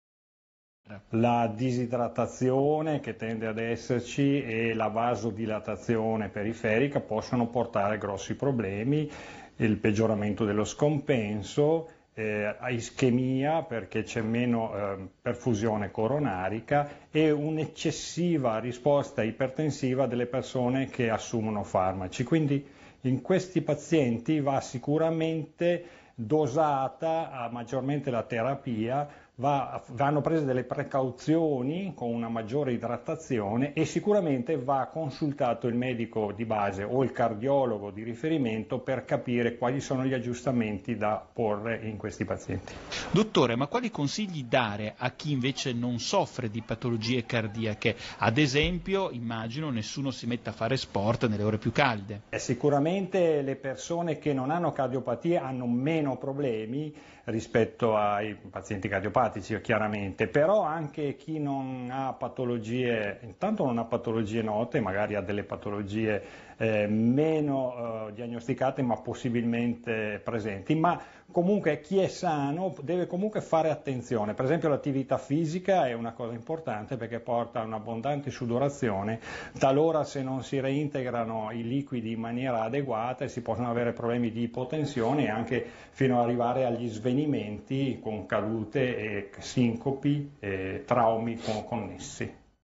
Caldo e cardiopatie: qualche consiglio
Intervista del cardiologo Giulio Molon a Telearena